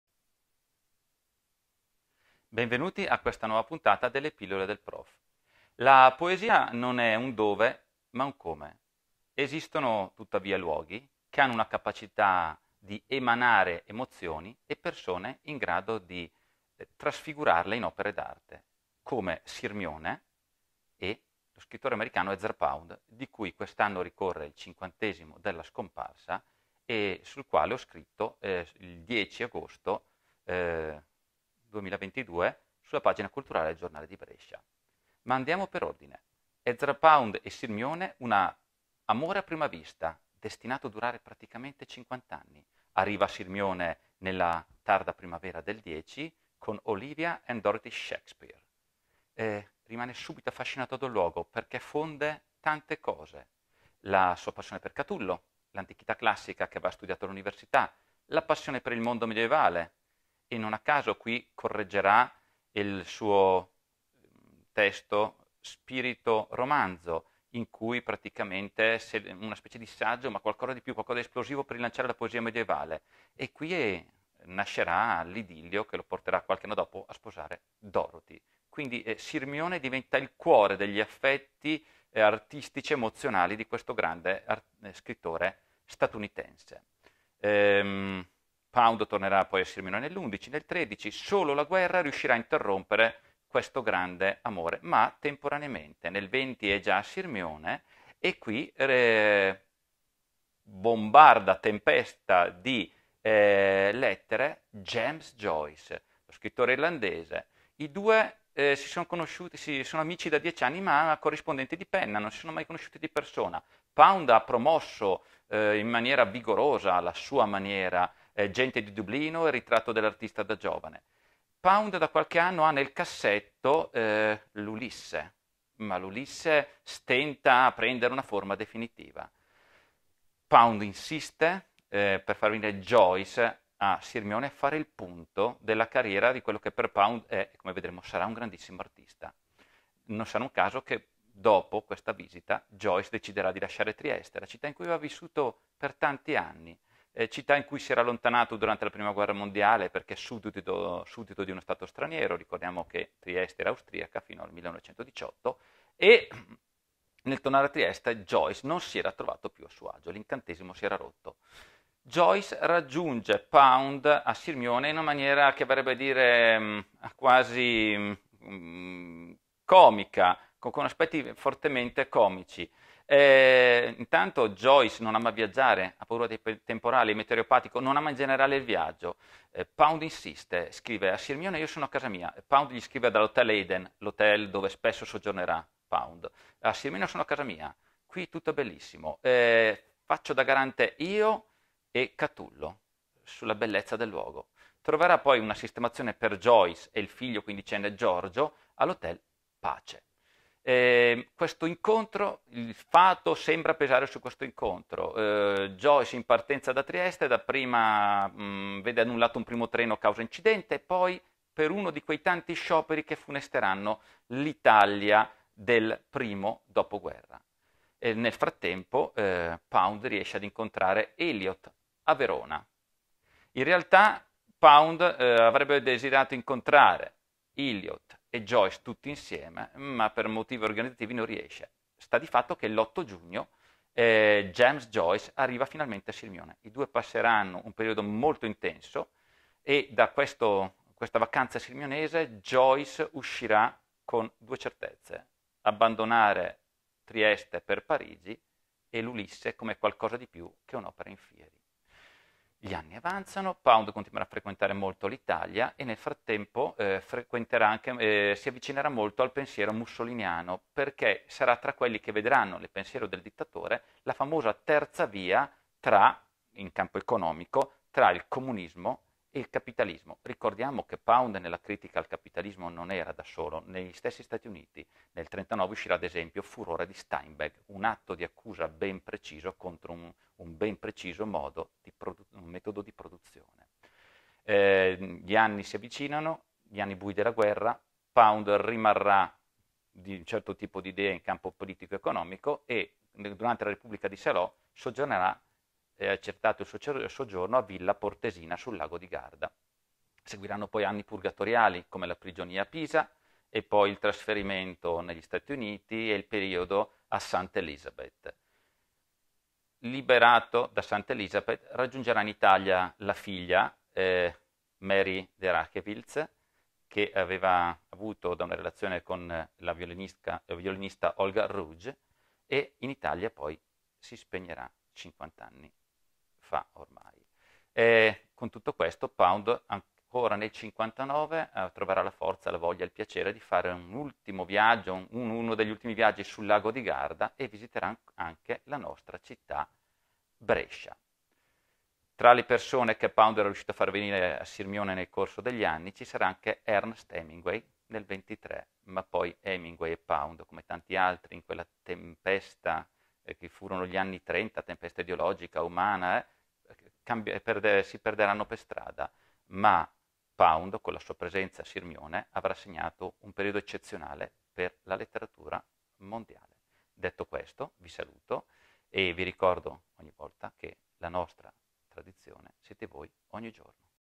Ezra Pound e James Joyce a Sirmione
A cinquant’anni dalla scomparsa di Pound e a un secolo dall’Ulysses di Joyce. Un focus approfondito sul mio articolo del 10 agosto 2022 sul Giornale di Brescia.